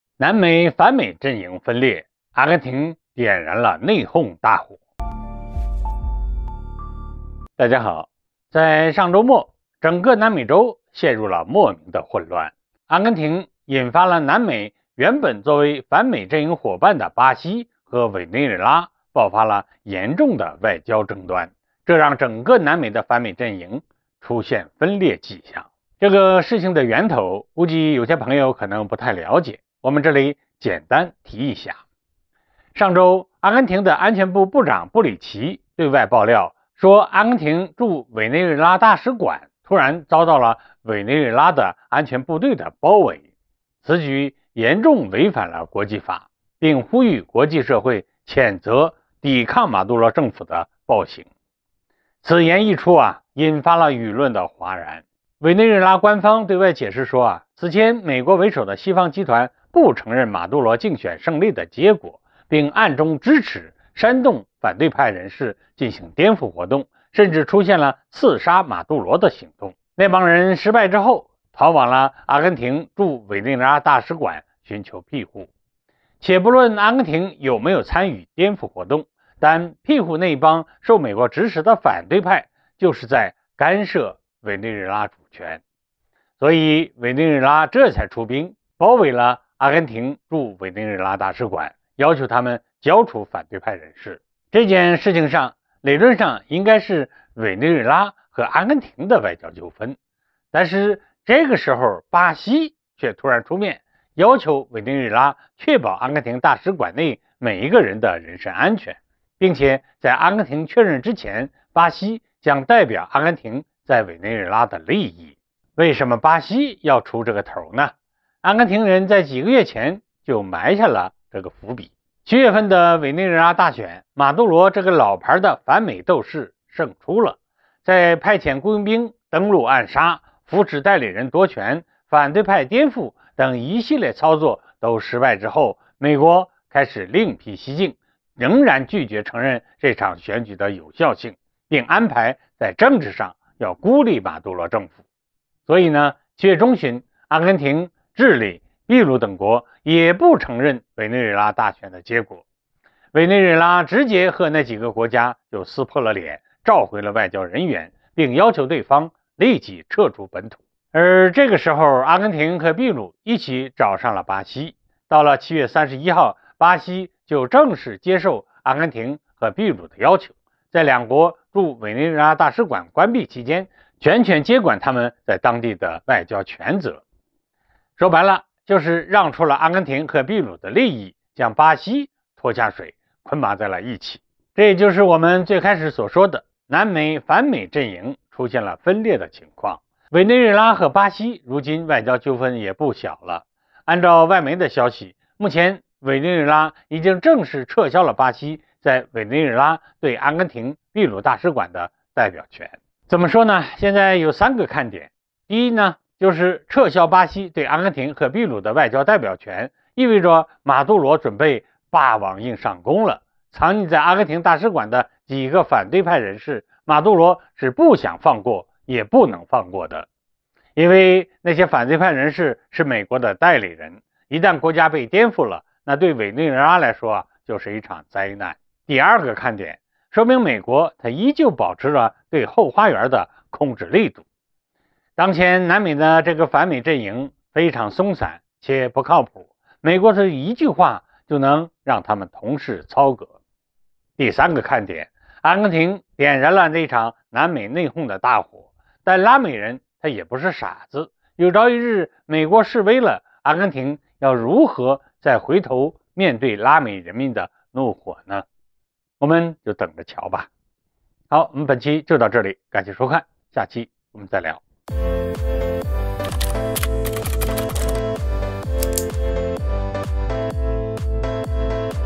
南美洲反美陣營內訌，為了阿根廷的事，巴西和委內瑞拉吵起來了
南美洲反美陣營分裂，阿根廷點燃內訌大火